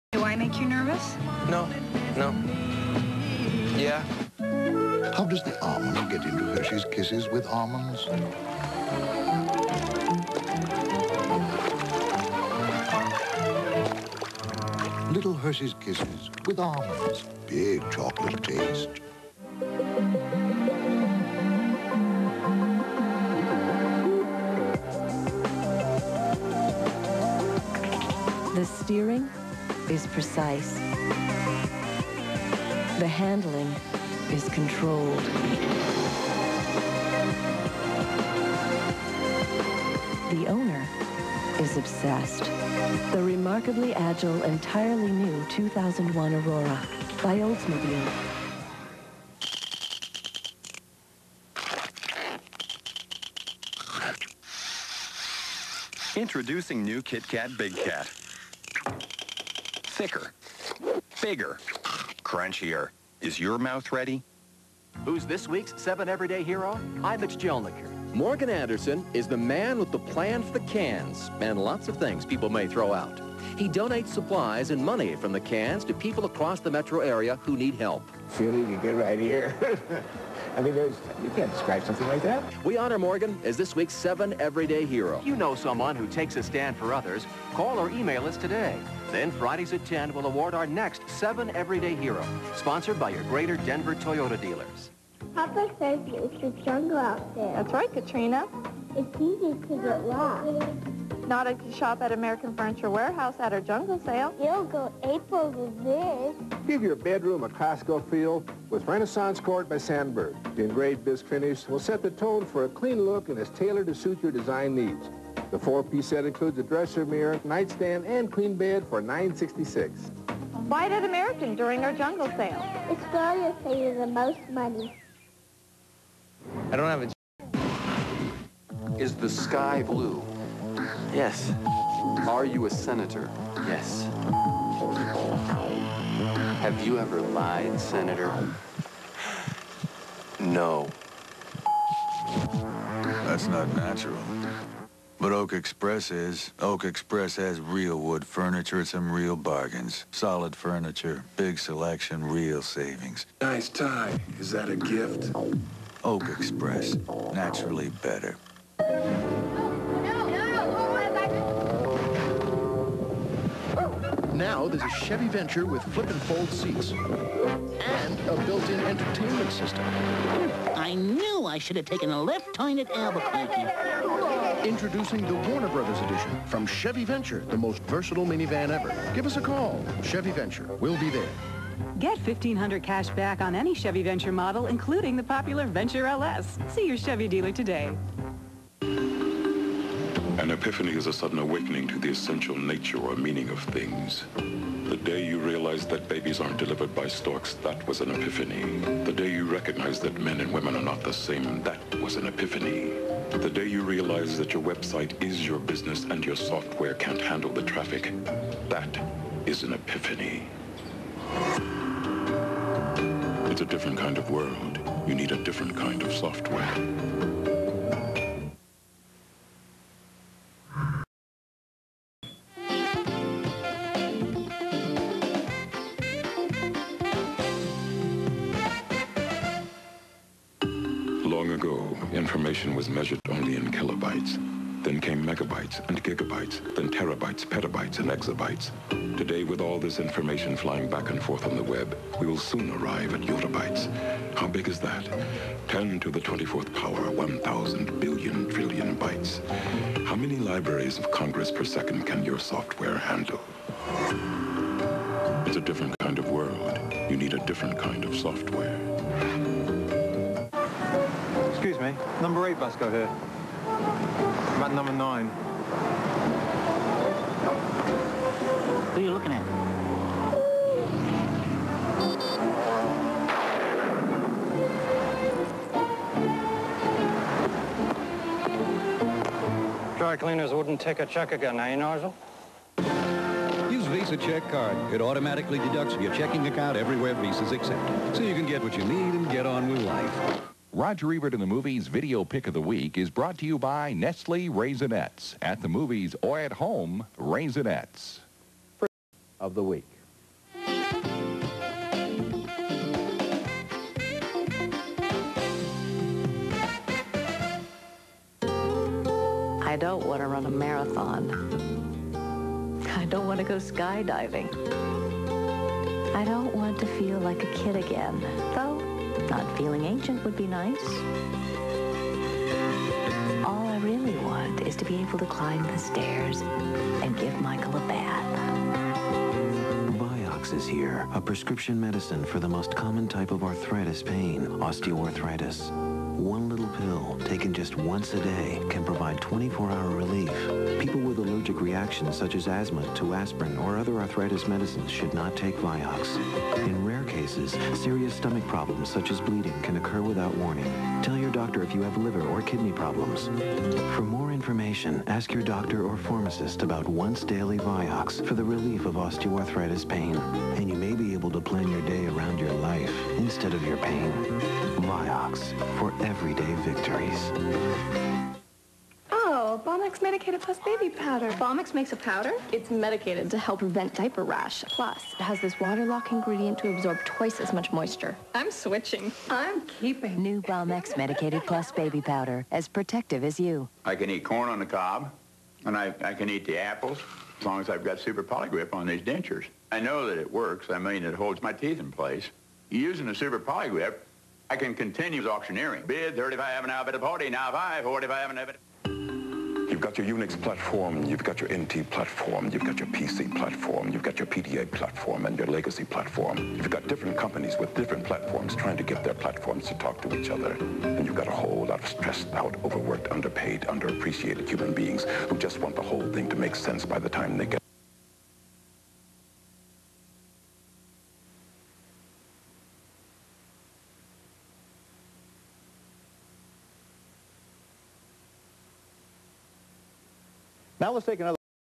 KMGH commercials, 6/18/2000
Aired during Roger Ebert & The Movies (with guest host Richard Roeper which would become the permanent co-host in a few months).

- Hershey's Kisses w/ Almonds
- Oldsmobile Aurora
- KitKat BigKat (Chattering Mouth)
- 7 Everyday Hero Award promo (Morgan Anderson)
- American Furniture Warehouse (Jungle Sale)
- Oak Express (Lie Detector)
- Chevy Venture
- IBM (WebSphere Software)
- Now in Theaters...
- IBM
- Visa Check Card (Dry Cleaners)
- New on Video...
- Vioxx
- Balmex Medicated Plus
- Super Poligrip (Auctioner)
- IBM (partial - with technical difficulties/dead air)